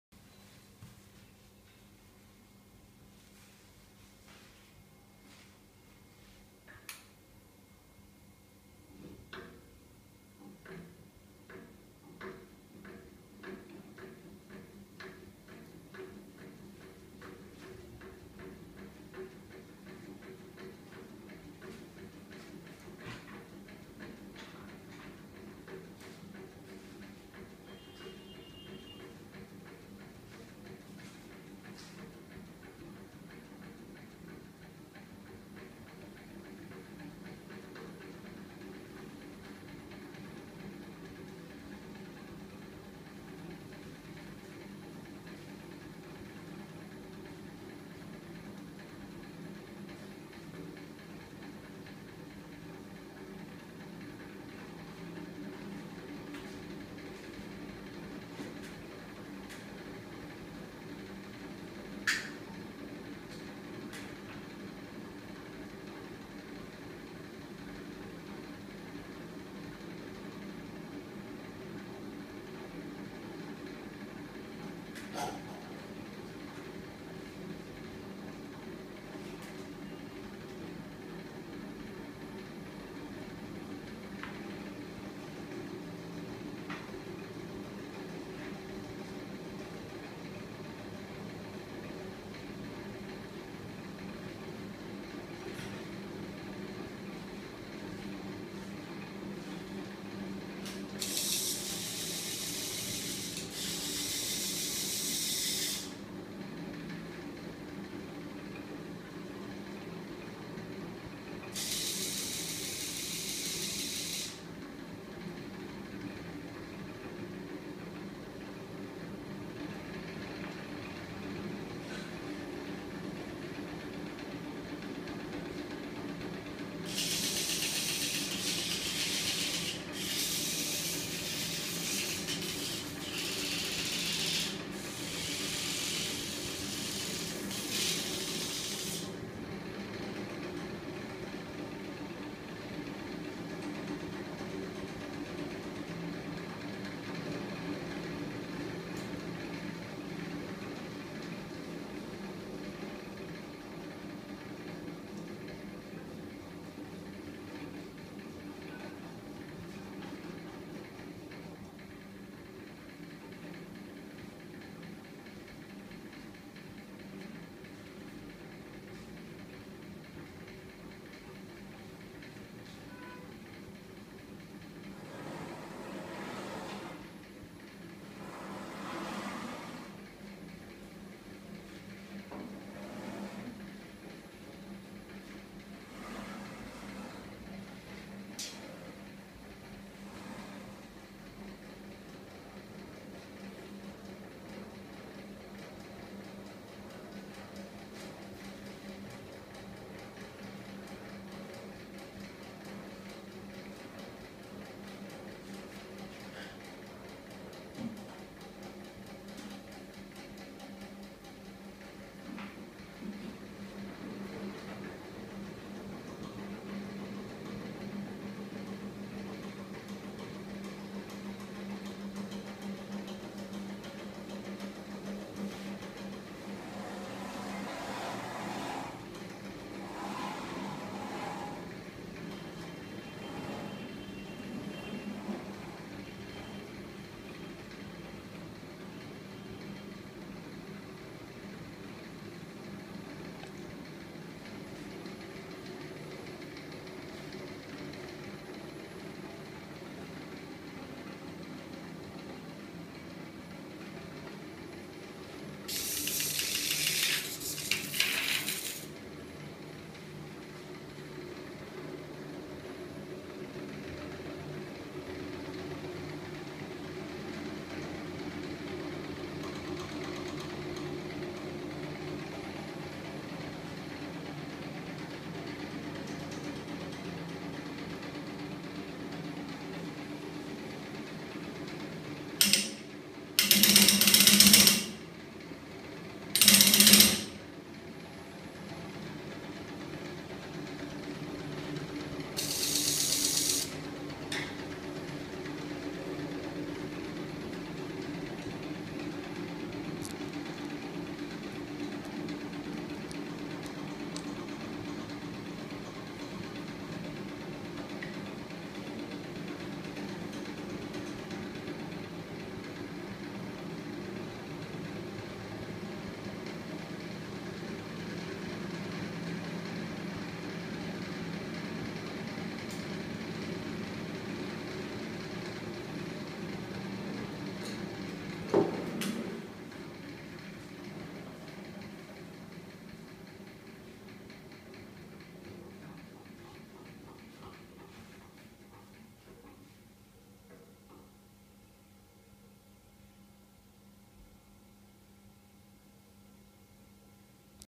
Airplane magnet motor (Low Voltage magnet Device) magnet motor engine
The process of building a magnetic machine
ornithopter engine
This engine used 12v battery and 24v electric motor!